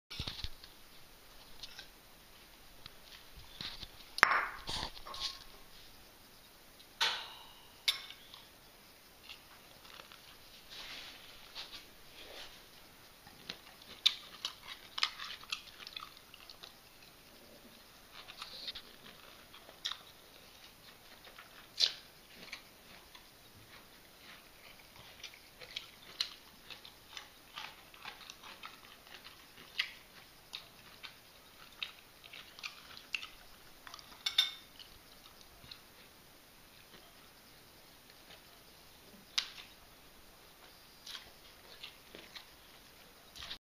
Bow Enjoys Brussels Sprouts
Some people don't like Brussels Sprouts. Bow is not one of them.

He had a whole bowl full for dinner last night. Then I offered him two more, which were leftover from somebody else's meal. This is a video of him enjoying the extra sprouts.

These Brussels sprouts have been boiled, but only  lightly, so they are still quite crunchy.